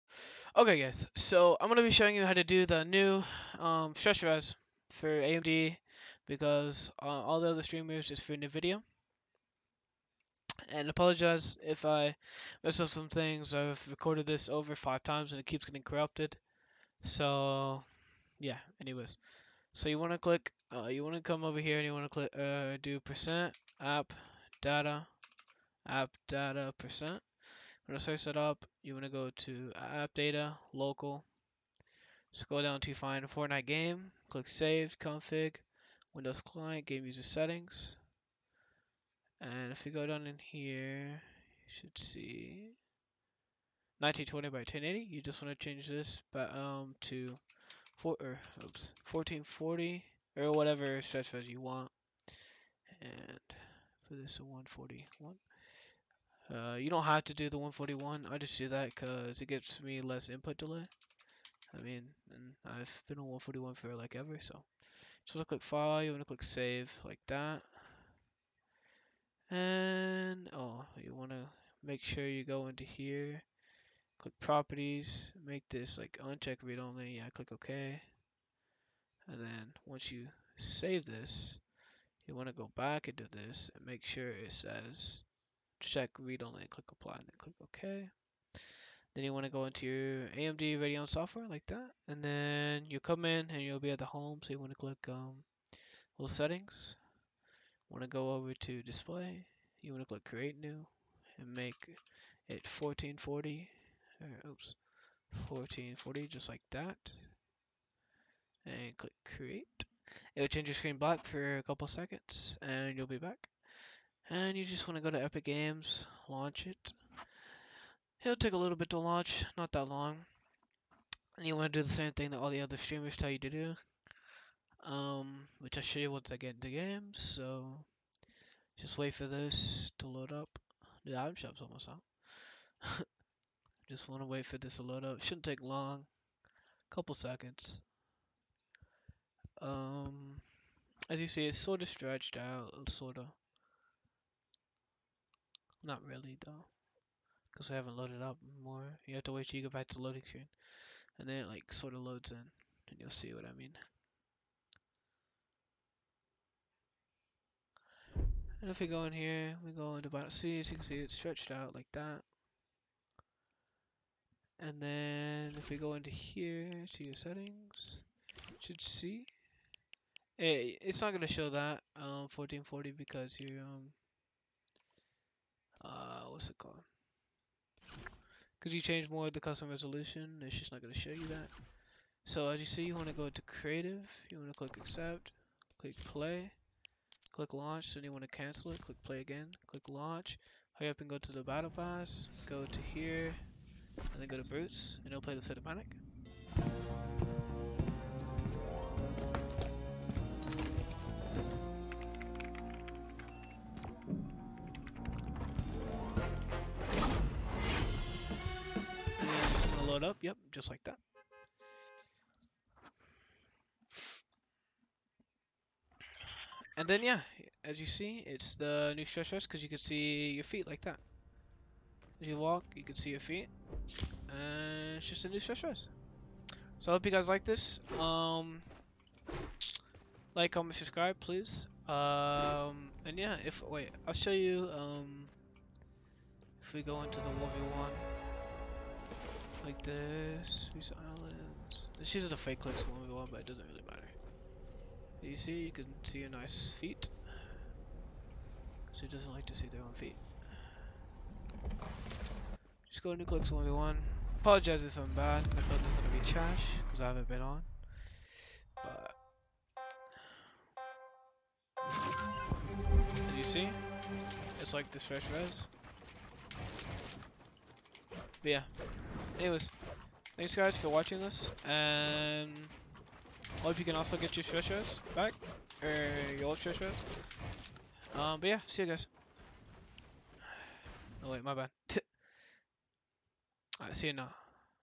How to get the new stretched resolution for amd gpus
In this video I just show you how to get the old stretched res, but on amd gpu's hope this help you and like, comment, and subscribe please and thank you.